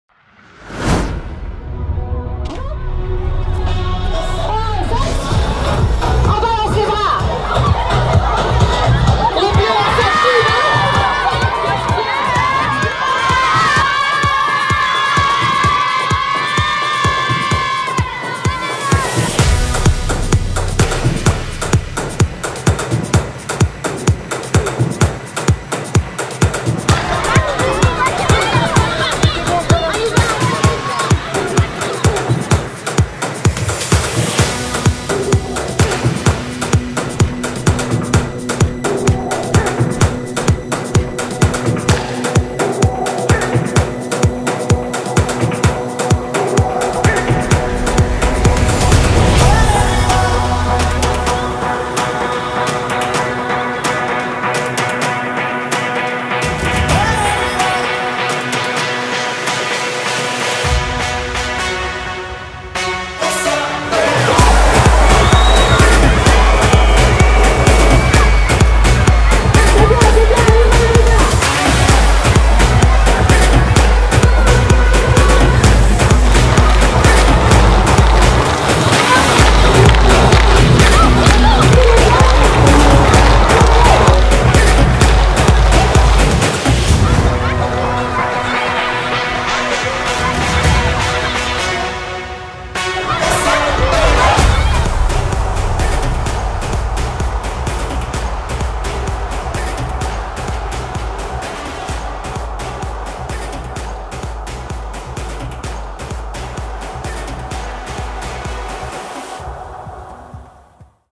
6P en fête - Les élèves lausannois célèbrent la journée olympique
Magnifique chorégraphie des élèves lausannois de 6P lors de la Journée Olympique du 23 juin 2017